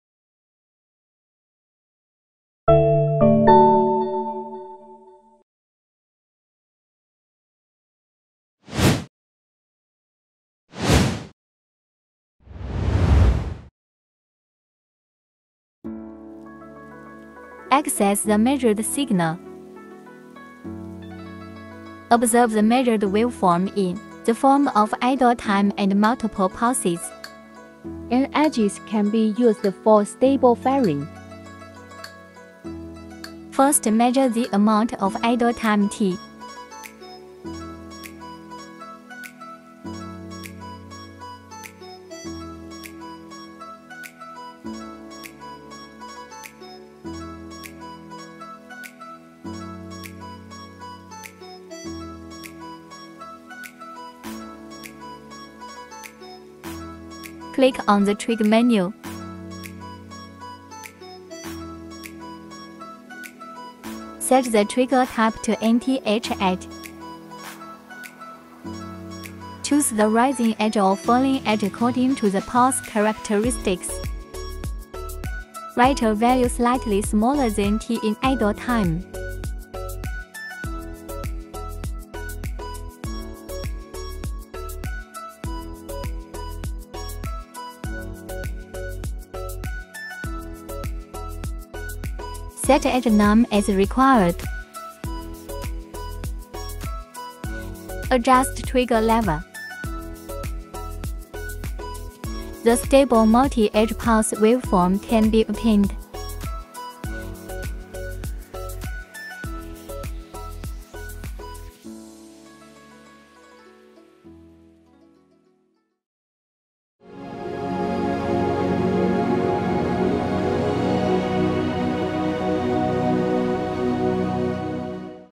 Introducing the N Edge Triggers Function of UNI-T UPO1000CS Series Digital Oscilloscopes
This video introduces the N Edge Triggers Function of the UNI-T UPO1000CS Series Digital Oscilloscopes.